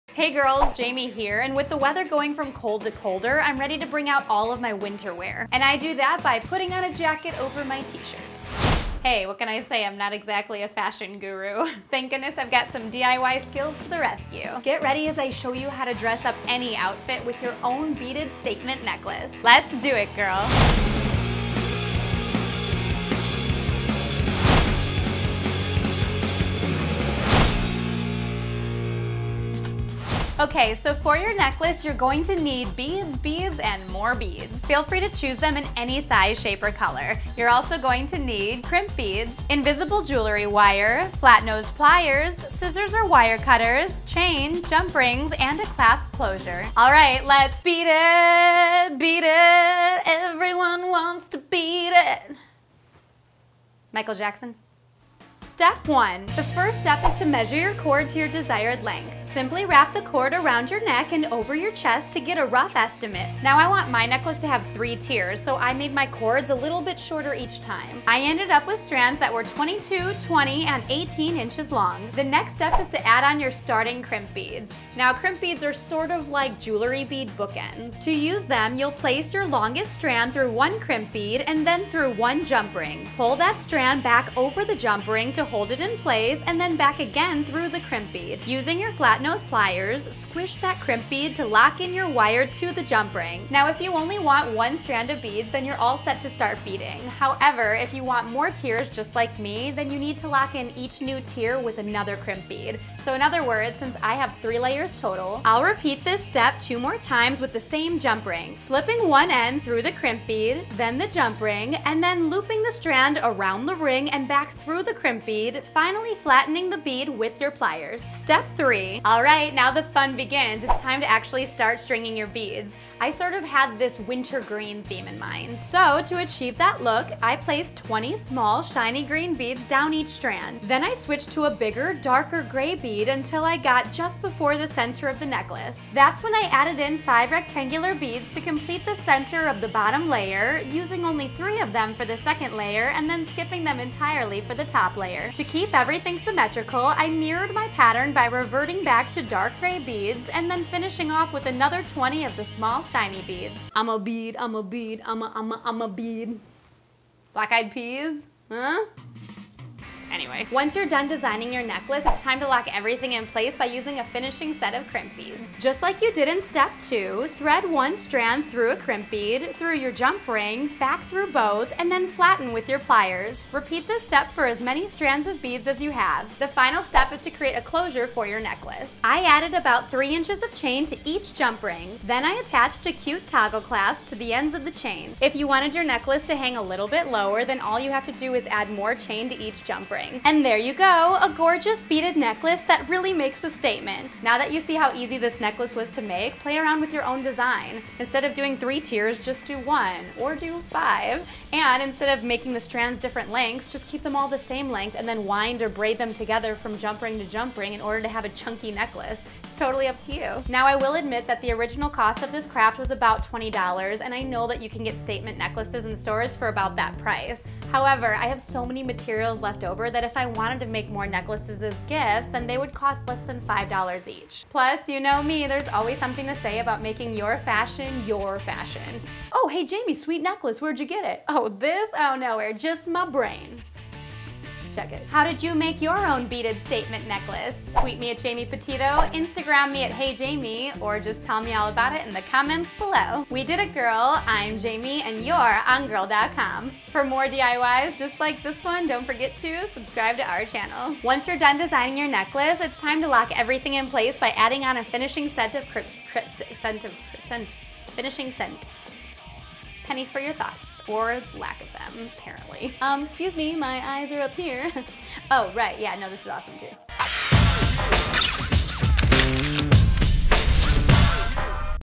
DIY Statement Necklace! - Do It, Gurl
Do It, Gurl

"Statement Necklace"

In this episode of Do It, Gurl, Jamie is showing how to make a beaded statement necklace! This DIY tutorial  is great if you are looking to add some new pieces to your jewelry collection or if you want to create unique holiday gifts. Watch now to see how Jamie makes her statement necklace!

Supplies:
Beads
Crimp Beads
Invisible Jewelry Wire
Flat-Nosed Pliers
Scissors or Wire Cutters
Chain
Jump Rings
Clasp Closure

Steps:
Step 1: Measure Your Cord
Step 2: Add Starting Crimp Beads
Step 3: Bead Your Strands
Step 4: Add Ending Crimp Beads
Step 5: Add Closure